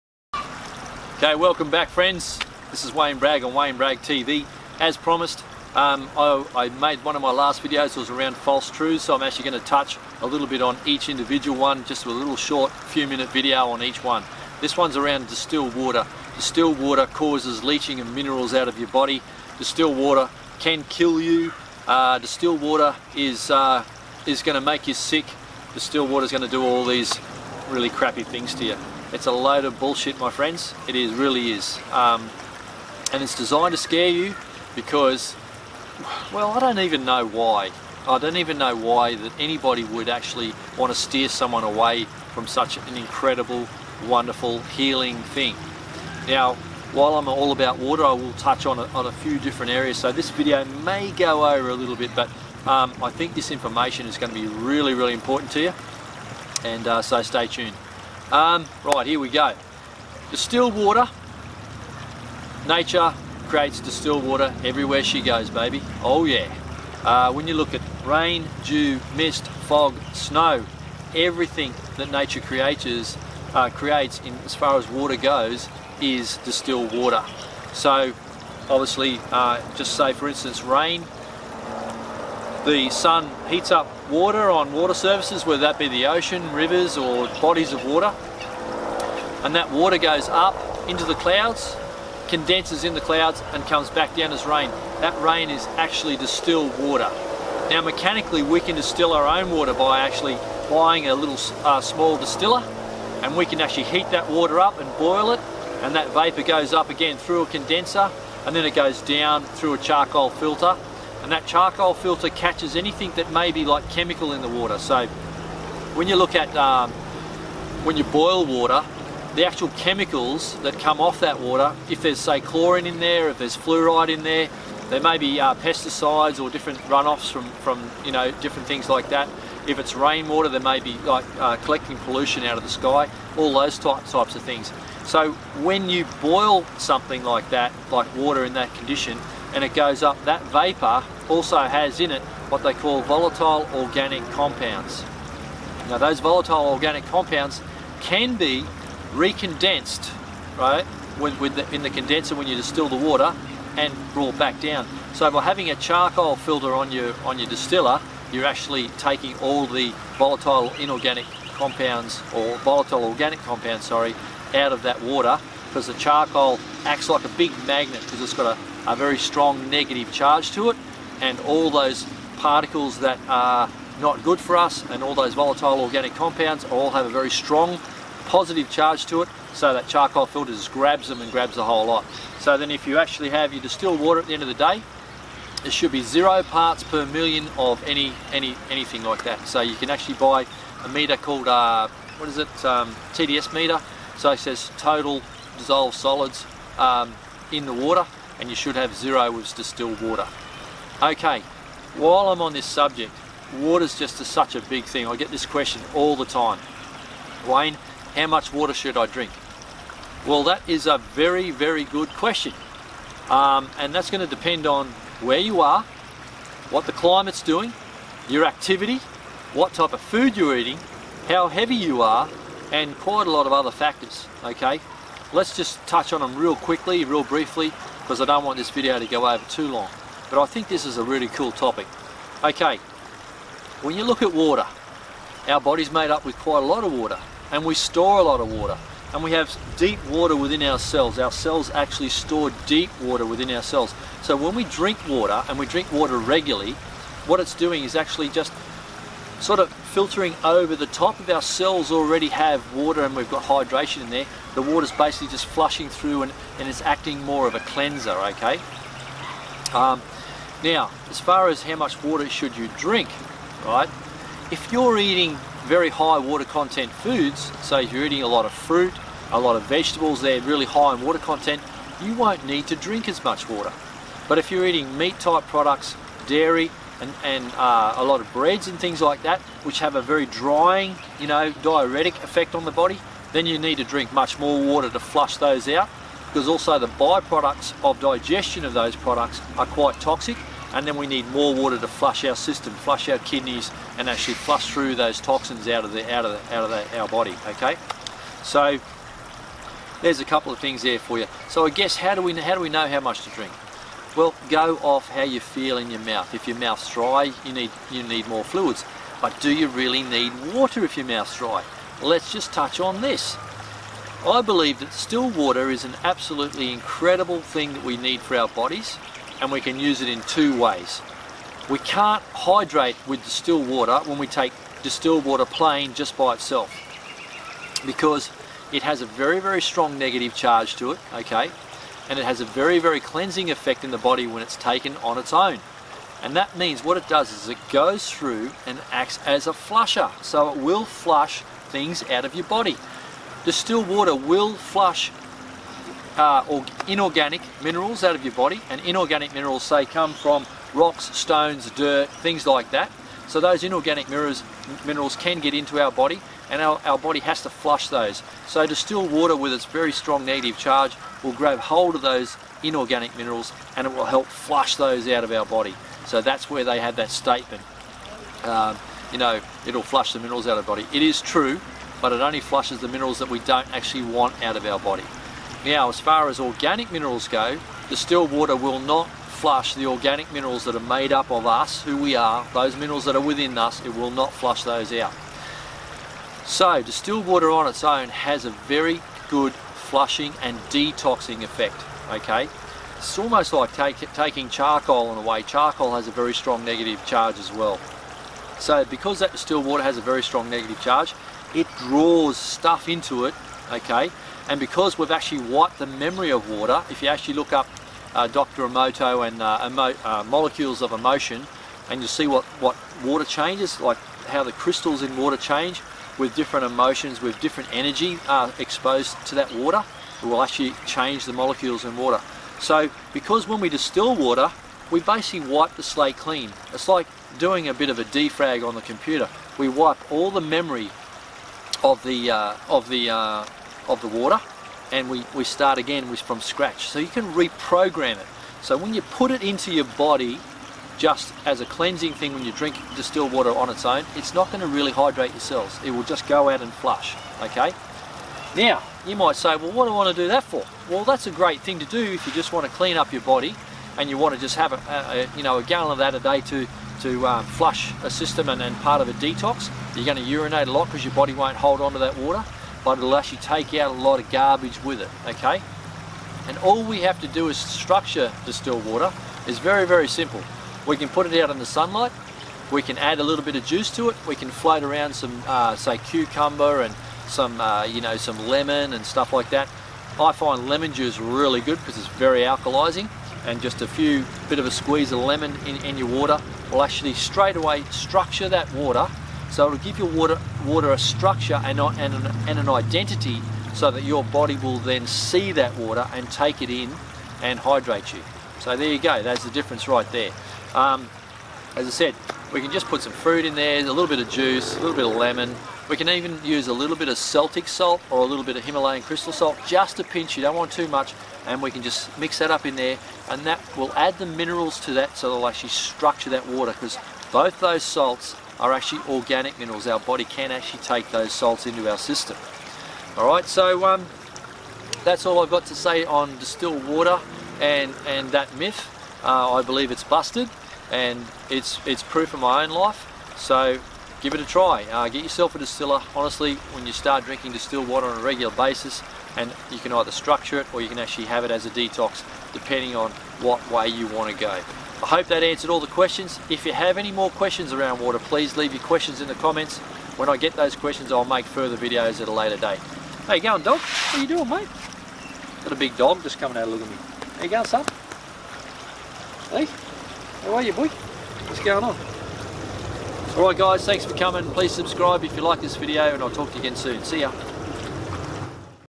The Truth about Distilled Water
Wayne explores if Distilled Water is really good for us.